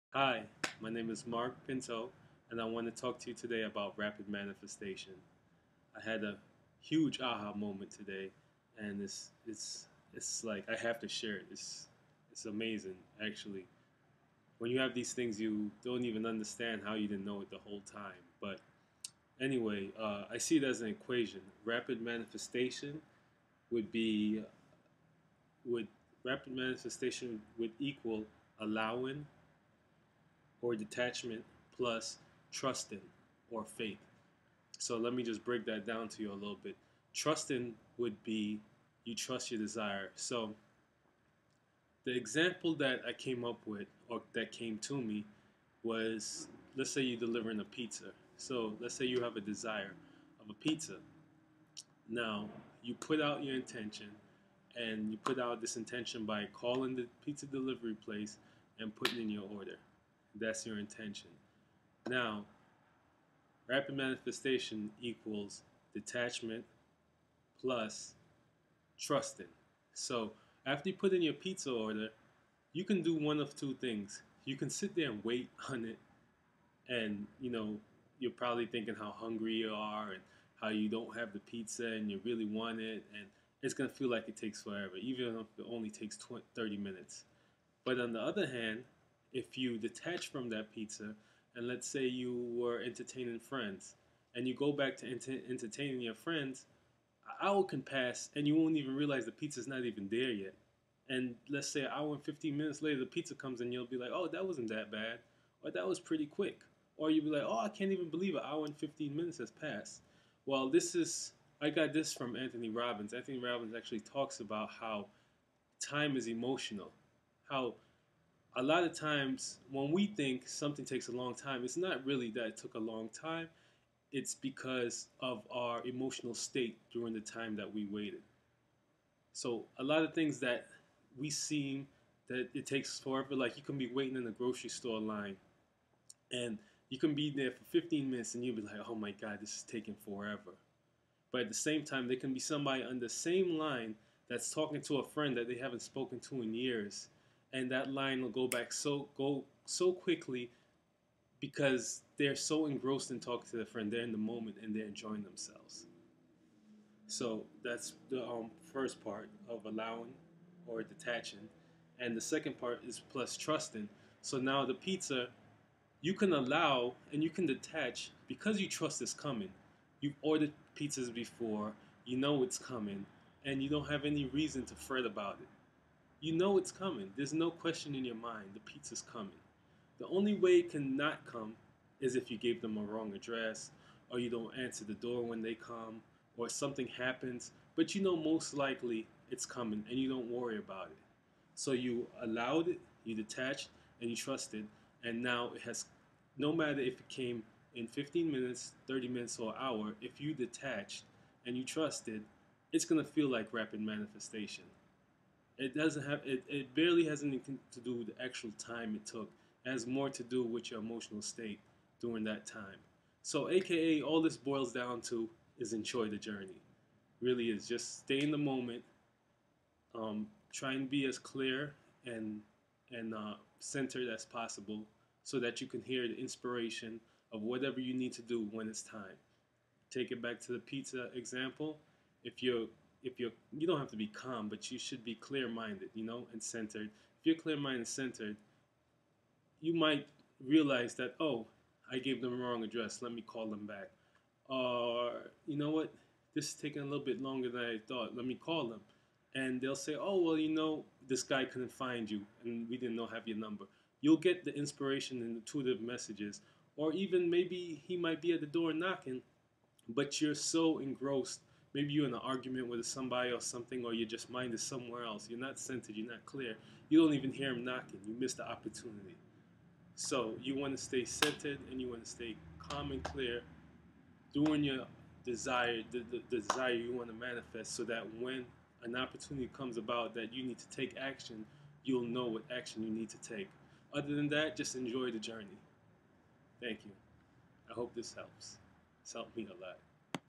Instant Manifestation: How to Get Things as Fast as possible
In a major aha moment I share with you the inspiration I received on creating  instant manifestations. Instant or  rapid manifestations are possible an happen to people who understand these keys or equation I received. I hope this information brings you as much clarity as it has for me. Thanks for you time and please "Like" if this has helped you in anyway!

Link to Enjoying the Journey by Abraham Hicks. BELOW